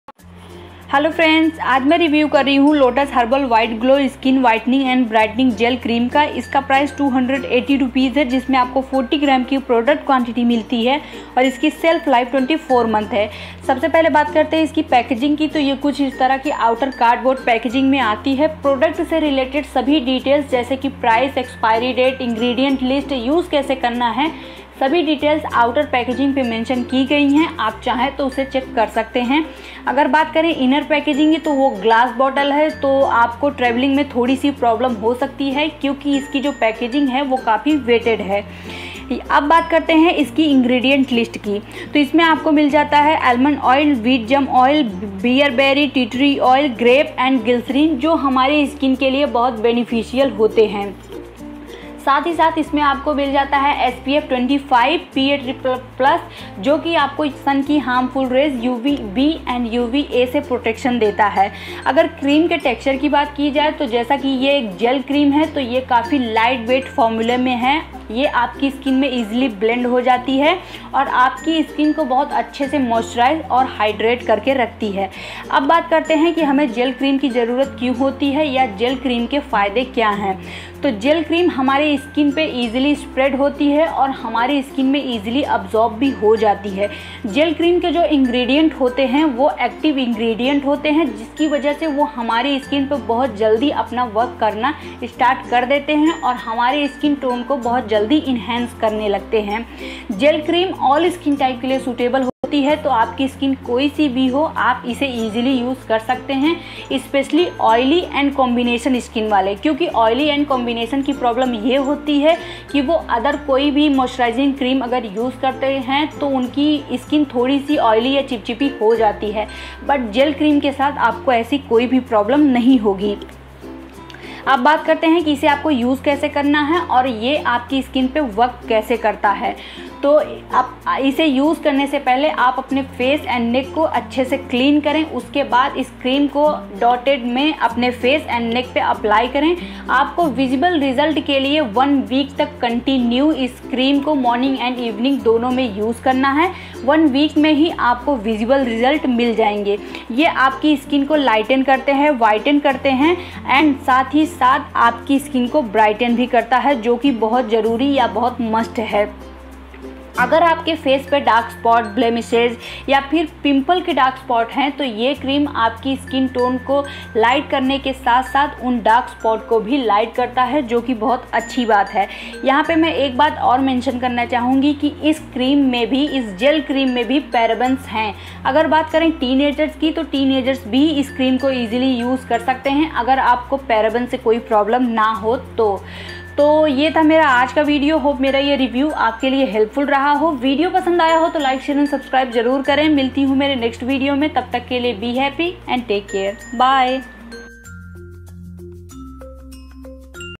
Lotus Herbals White Glow Whitening & Brightening Gel Cream l Honest Review l Tiny Makeup Update
Lotus Herbals White glow Whitening & Britening Gel cream ...
Lotus Herbals White Glow Whitening & Britening Gel Cream Review...

Price... 280/40gm
Self life.. 24 Month
SPF 25 PA+++
All skin type